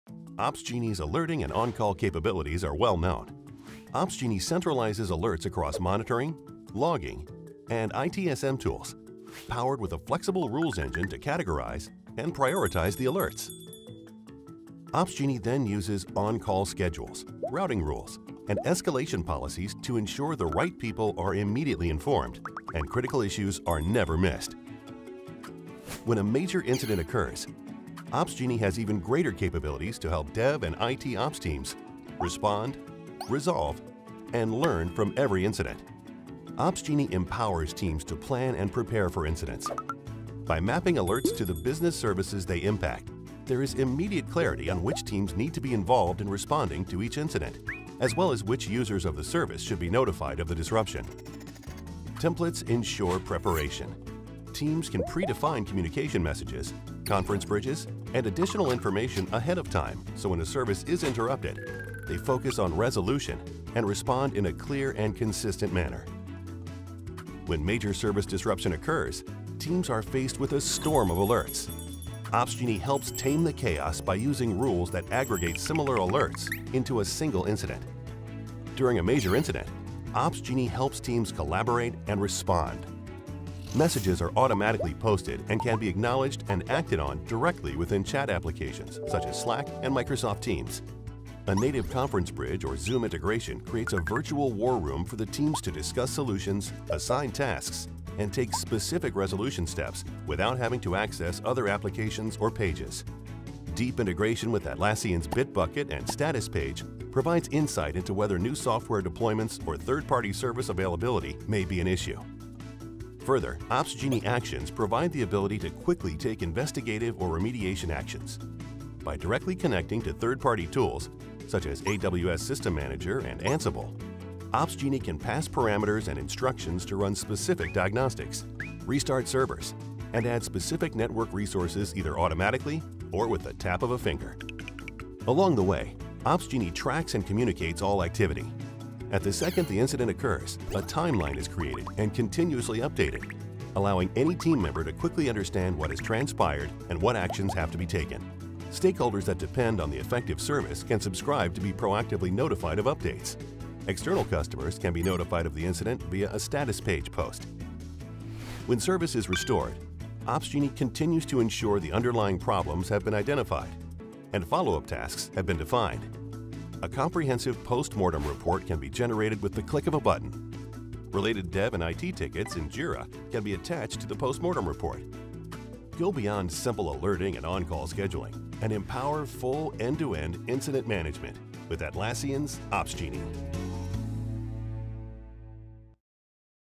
Incident Management with Atlassian's Opsgenie
Learn about how Opsgenie goes beyond simple alerting and on-call scheduling to empower teams' end-to-end incident management process. Respond, resolve and learn from every incident.